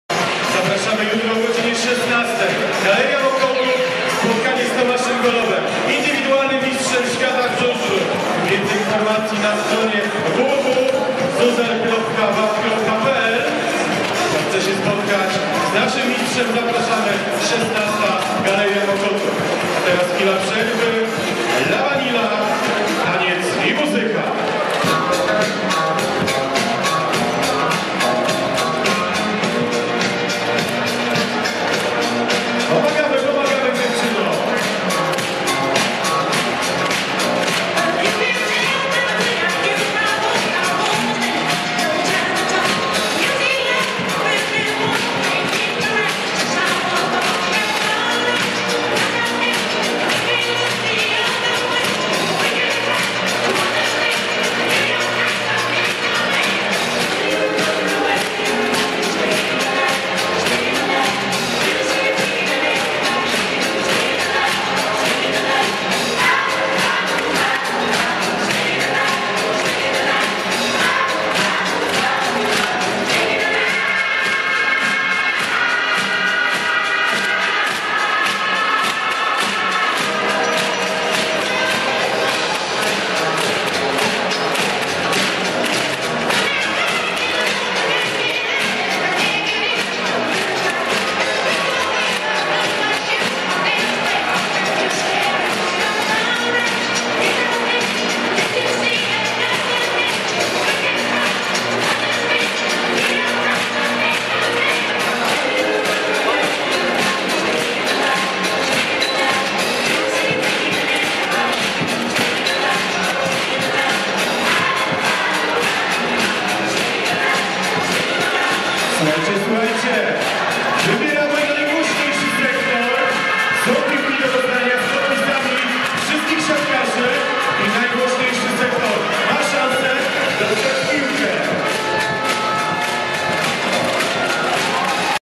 ZAPOWIEDZ TOMASZ GOLLOBA TORWAR
W trakcie meczu plusligi siatkarskiej AZS Politechnika Warszawa-ZAKSA Kędzierzyn Koźle zapowiedziano spotkanie związane z promocja książki Mistrza Świata na Żużlu TOMASZA GOLLOBA które odbędzie się w GALERII MOKOTÓW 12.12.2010r zorganizowane przez WARSZAWSKIE TOWARZYSTWO SPEEDWAYA